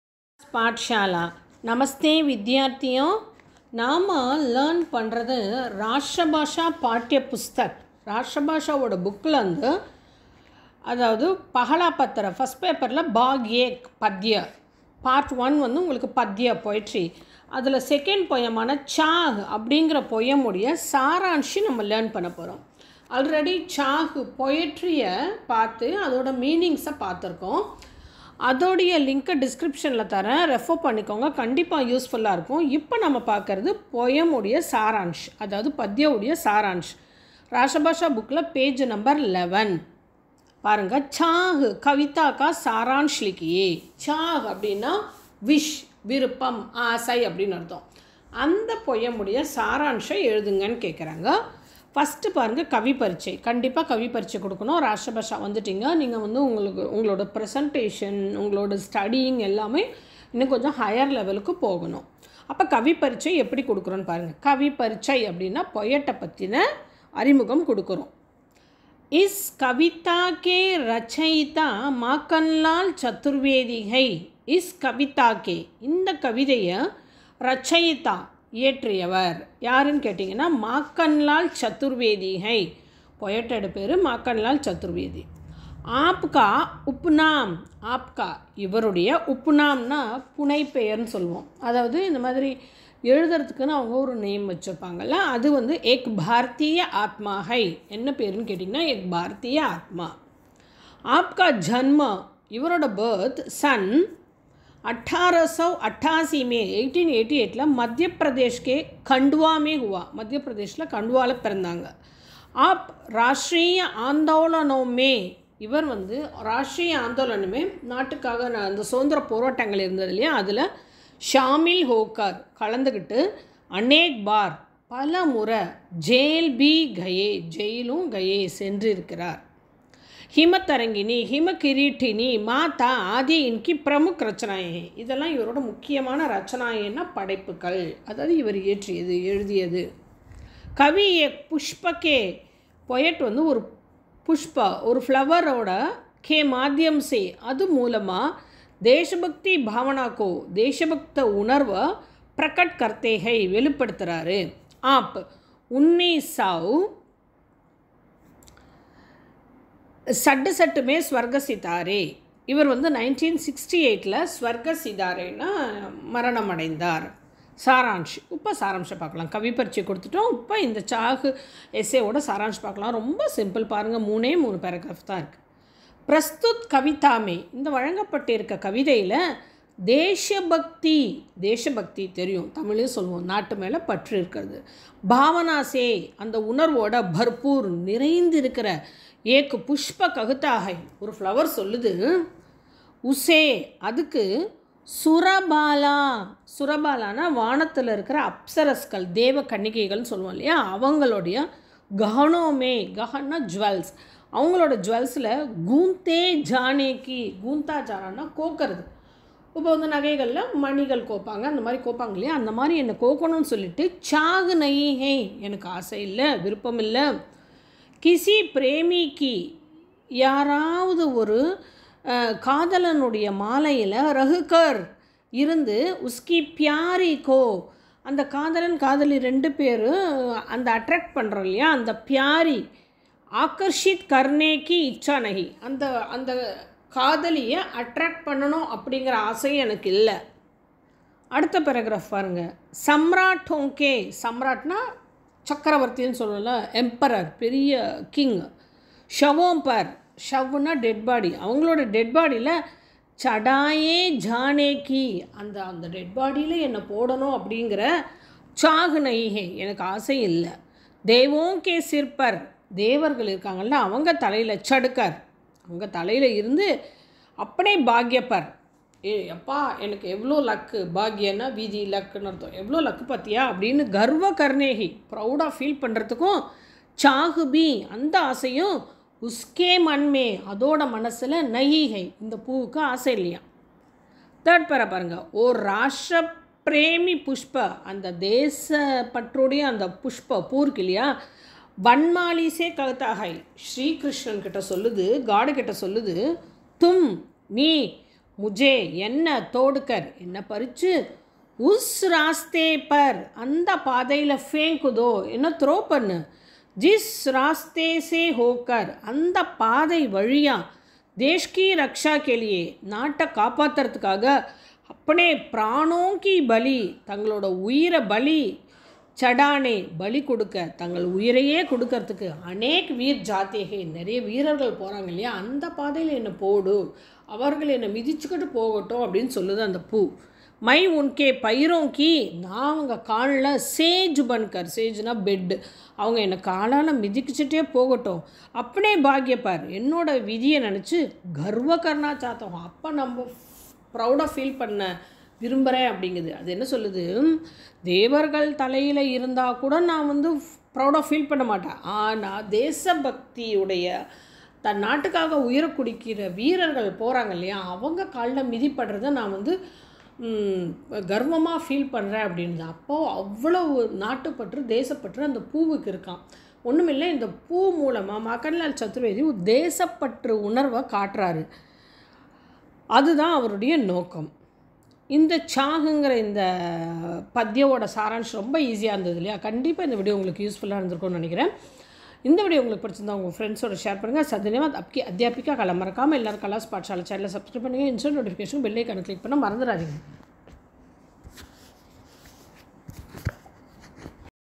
Rashtrabasha patya pustak - Padya 2 - Chaah - Saaransh in Tamil
नमस्ते विद्यार्थियों!

This video explains rashtrabasha patya pustak poetry chaah saaransh in Tamil. make use of it.

Comment us for any doubts.
Press Bell icon for instant notifications.

Admission is open for February 2024.

धन्यवाद!